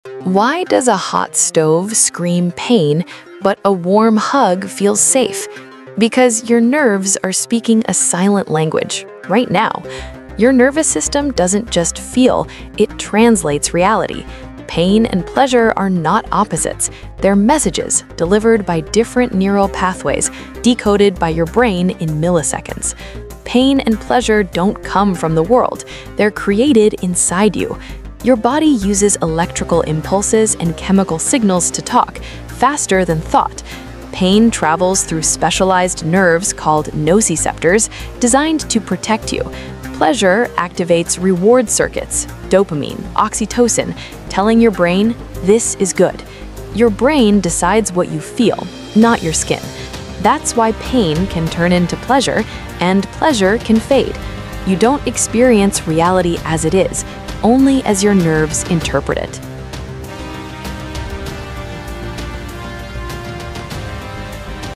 How Your Nervous System Translates Pain and Pleasure!!
Pain and pleasure feel like opposites—but they’re spoken in the same neural language.
Your nerves don’t just sense the world… they translate it.
What you feel depends on how your brain decodes the signal.

🧠 Reality isn’t experienced.
It’s interpreted.

Where facts meet fiction — and every theory has a story.
This is The Nexus Report.